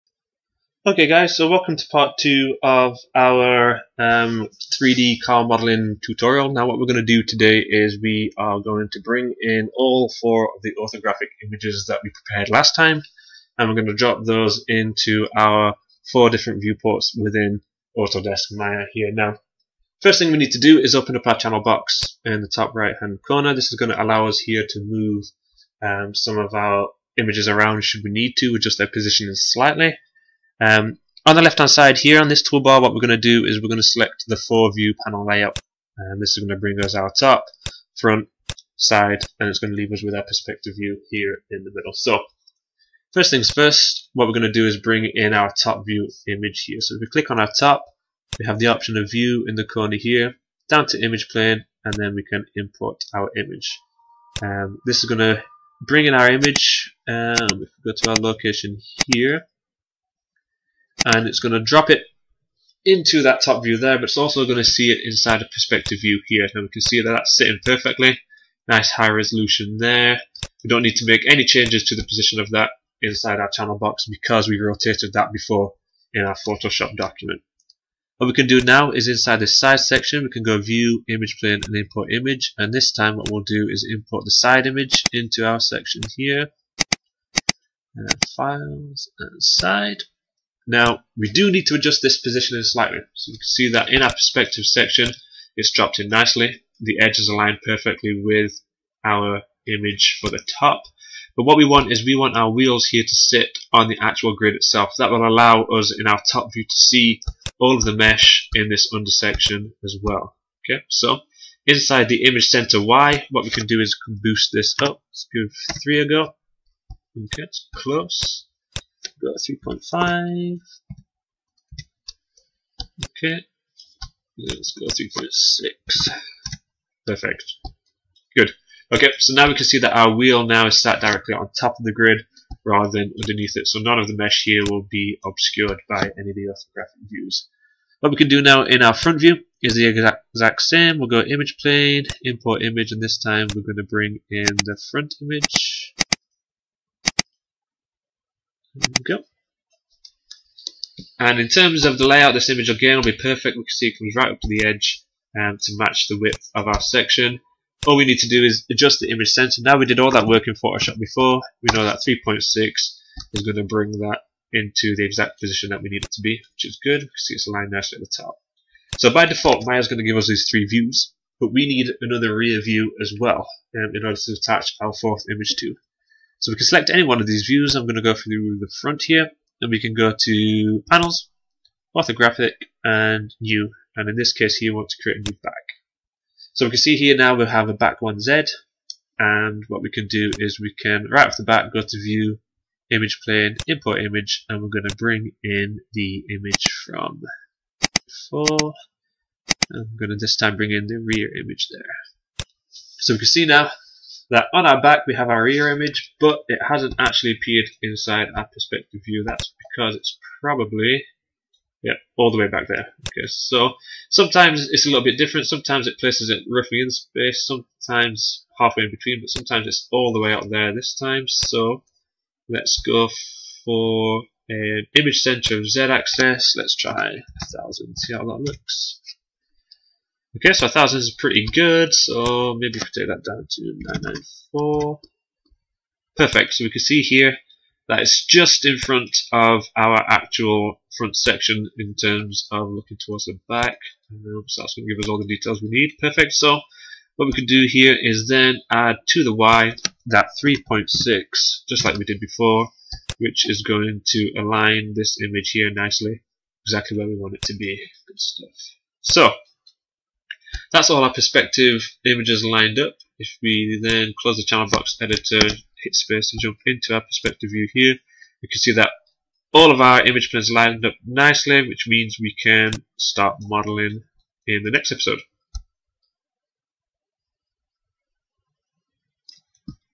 Creating 3D Blueprints in Maya and Photoshop pt. 2
This tutorial will cover the basics of creating 3D Blueprints in Adobe Photoshop before importing into Autodesk Maya.